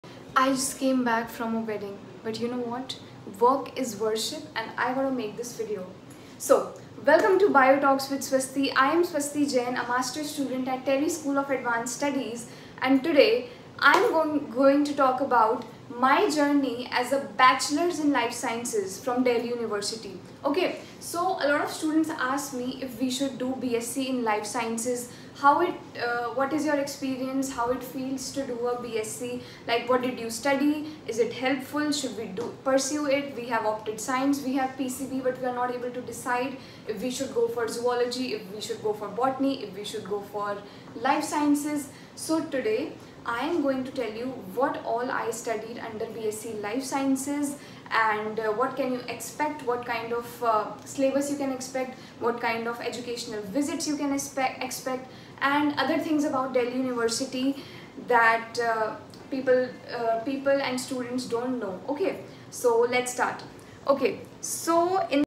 My Journey in B.Sc. Life Sciences at Delhi University | Subjects, Experiences at Dyal Singh College
🎓 Join me as I share my journey of pursuing a Bachelor’s in Life Sciences at Dayal Singh College, University of Delhi! 🌱

In this video, I’ll walk you through:
✅ The subjects and specializations I studied over three years
✅ What it’s really like to be a life sciences student at DU
✅ My favorite topics and the challenges I faced
✅ Tips and insights for future students considering B.Sc. Life Sciences

Whether you’re planning to study life sciences, already in college, or just curious about what this course offers — I hope my experience helps and inspires you! ✨

📍 Dyal Singh College, University of Delhi
📚 Course: B.Sc. Life Sciences

💬 Drop your questions or share your own college experiences in the comments!